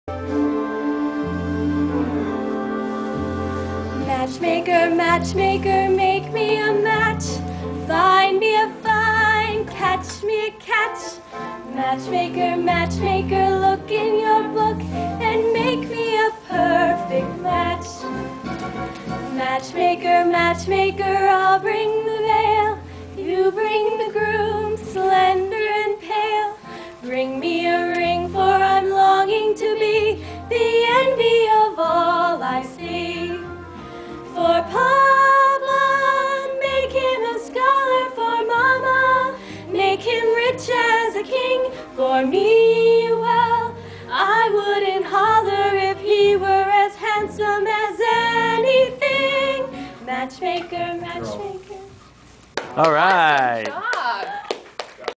Rachel and Amanda Barret
Singing on the morning show at the WMDT 47 studio in Salisbury MD